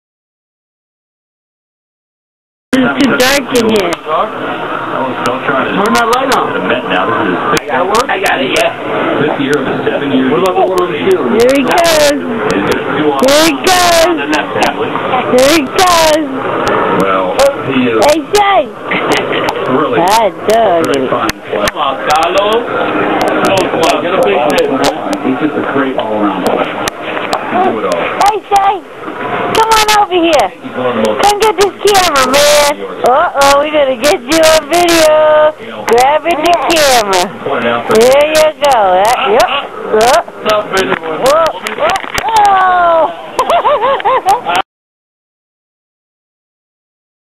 No More Papparrazzi!!!!
Shea takes his crawling serious.  He does not want to be on YouTube!